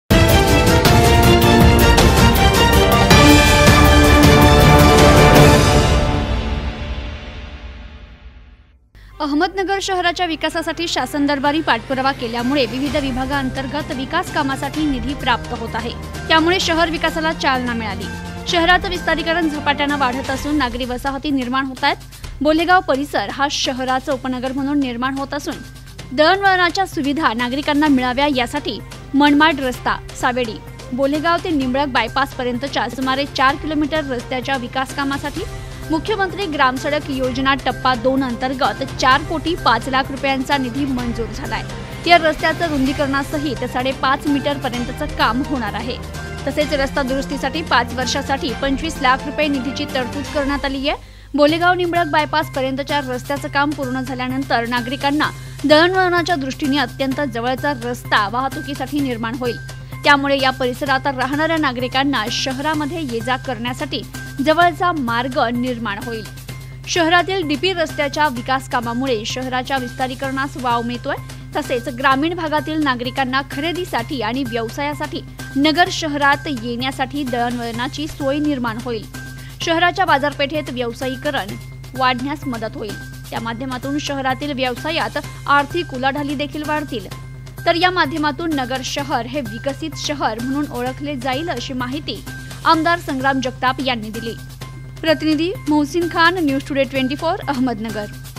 अहमदनगर : आमदार संग्राम जगताप यांच्या पाठपुराव्याला यश
मुख्यमंत्री ग्राम सडक योजना टप्पा अंतर्गत मनमाड रस्ता बोलेगाव ते निंबळक बायपास रस्त्याच्या विकास कामासाठी चार कोटी रुपयांचा निधी मंजूर